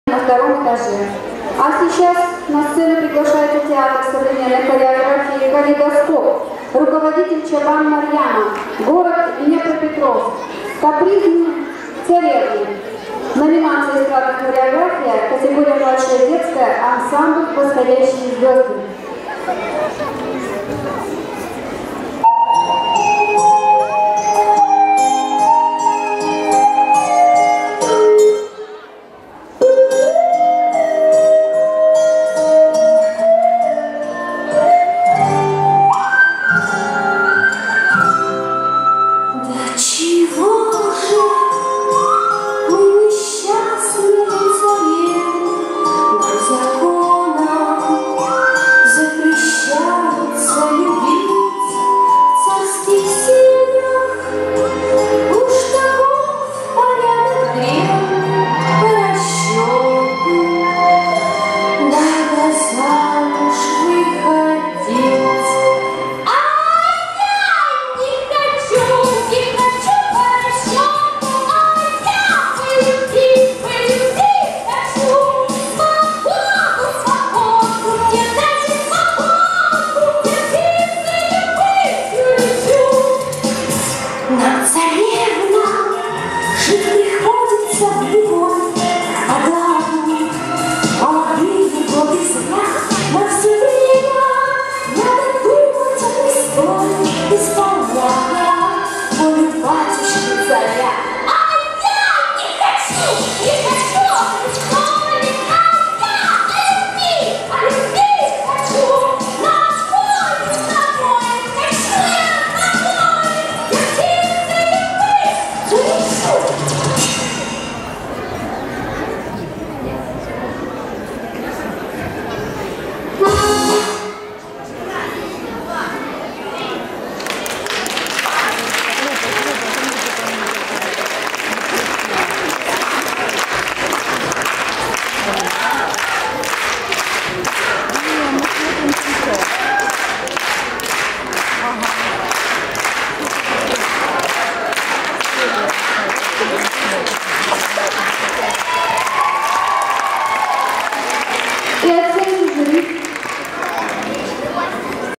Хореографический коллектив "Калейдоскоп" Лауреат 2 степени VII Международного фестиваля "Дни Европы"
VII Международный многожанровый фестиваль - конкурс
"Дни Европы"В МАЕ 2015 ГОДА
Театр современной хореографии "Калейдоскоп"
 Руководитель Чабан Марьяна
 г. Днепропетровск
"Капризные царевны"